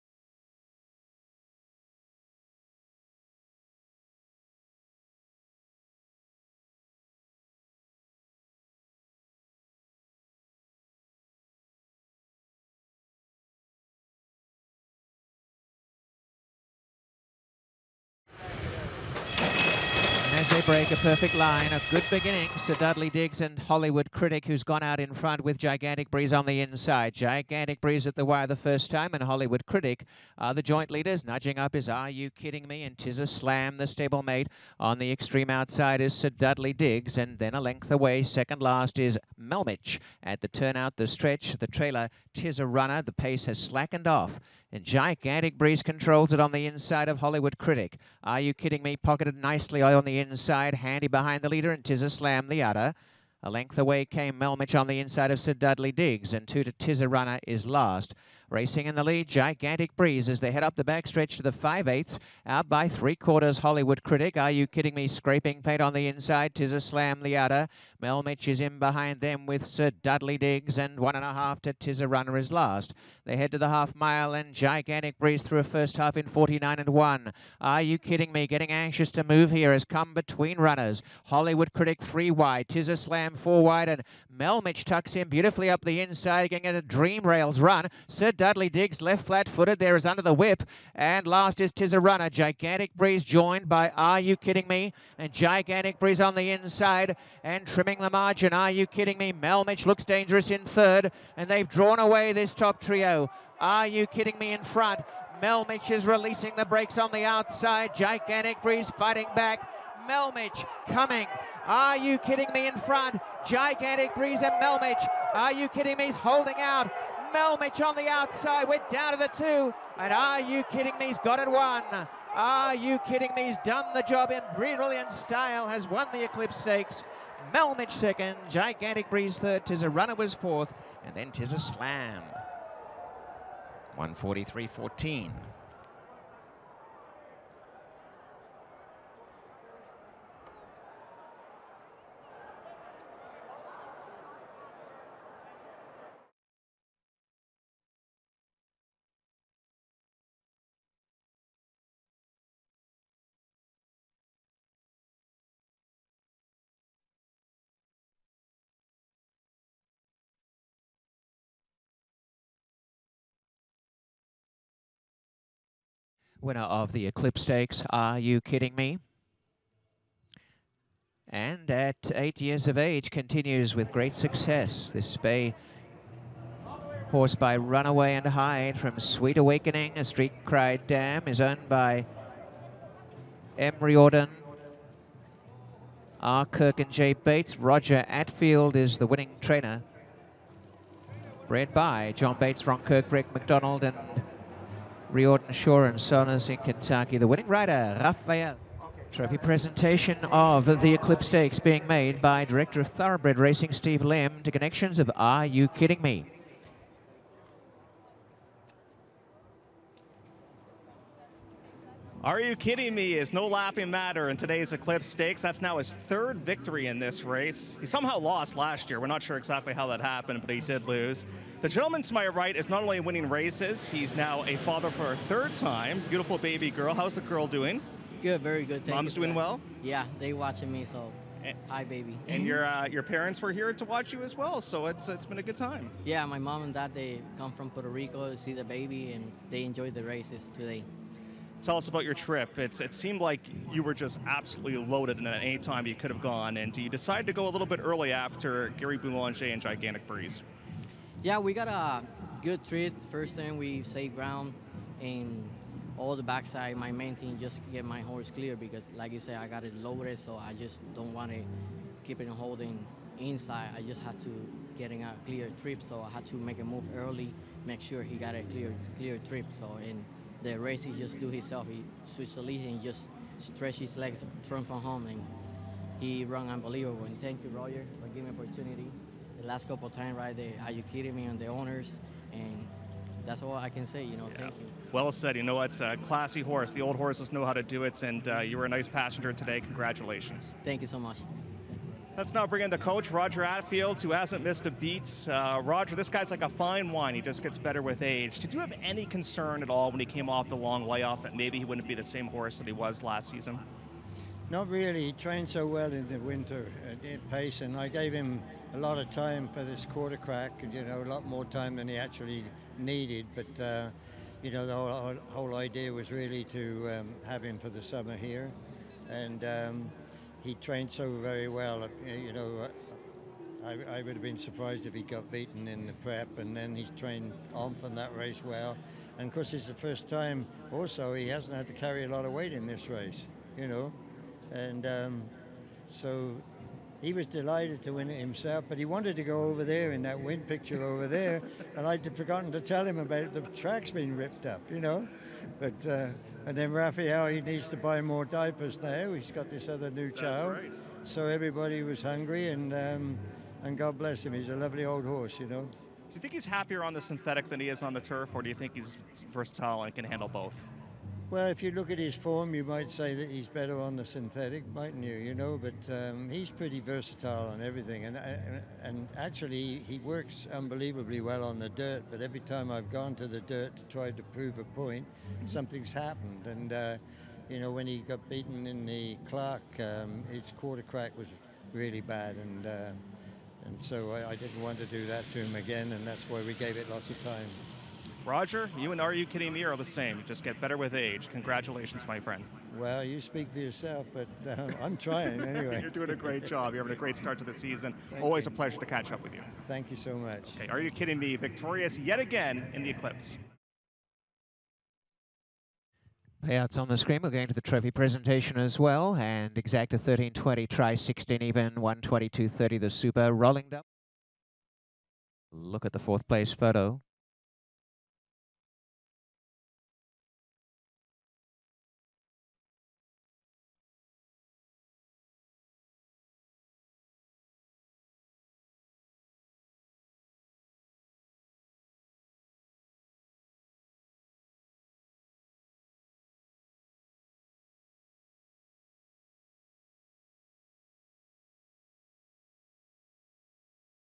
2018 Eclipse Stakes (Grade II): Woodbine, May 26, 2018 - Race 8
$175,000 Guaranteed purse.

Melmich - Eurico Rosa Da Silva
Gigantic Breeze - Gary Boulanger
Tizzarunner - Alan Garcia
Are You Kidding Me - Rafael Hernandez
Hollywood Critic - Catherine Sutherland
Tiz A Slam - Patrick Husbands
Sir Dudley Digges - Luis Contreras

Eclipse Stakes (GII). Race 8 from Woodbine, May 26, 2018